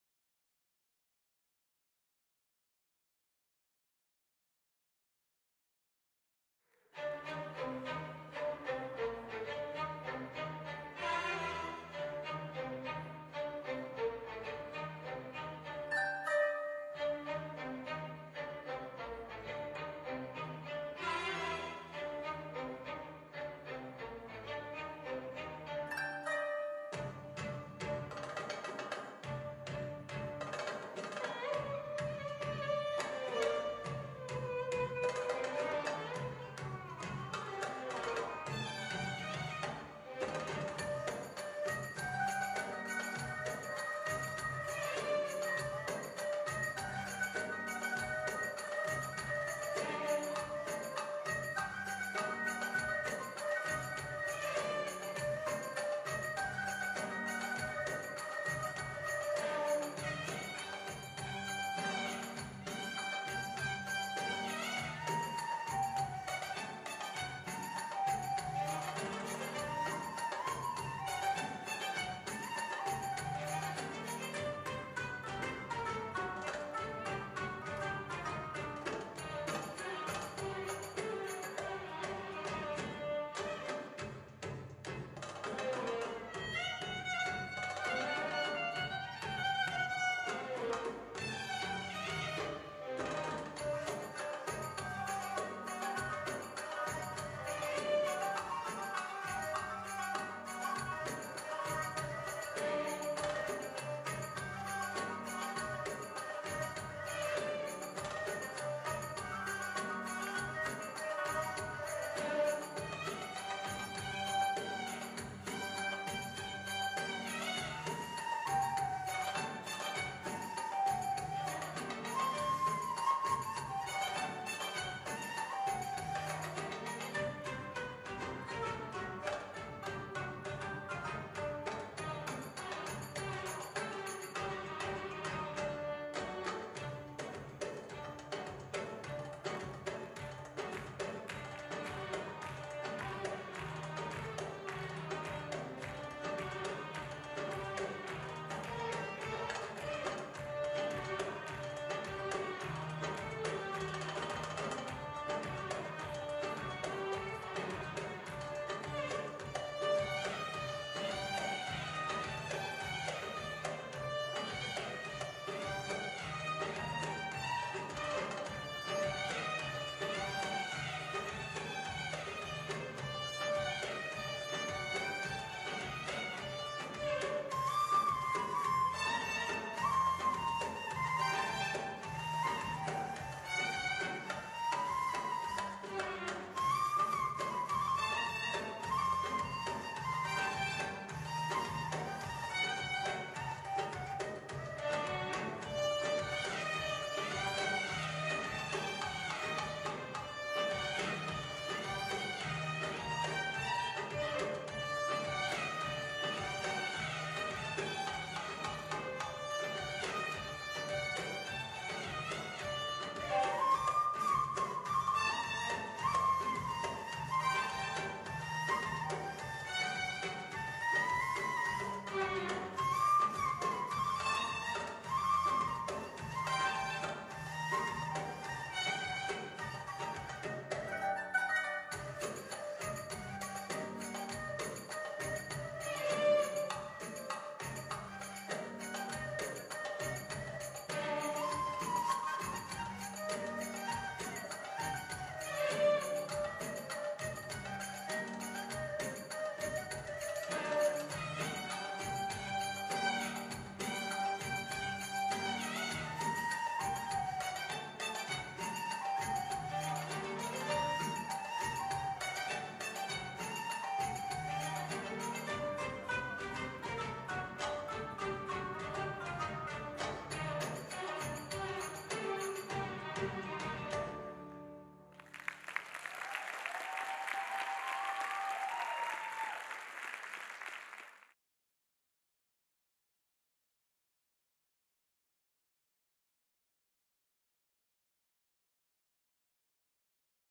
Oriental Fairytales 2018 - skola orijentalnog plesa Aruena - srednja grupa
Students from Aruena oriental dance studio performing during Oriental Fairytales Festival 2018 in Belgrade organized by Aruena.
Choreo by Aruena...
Ukoliko zelite da nam se pridruzite na casovima dobrodosli! Info potrazite na nasem web site-u.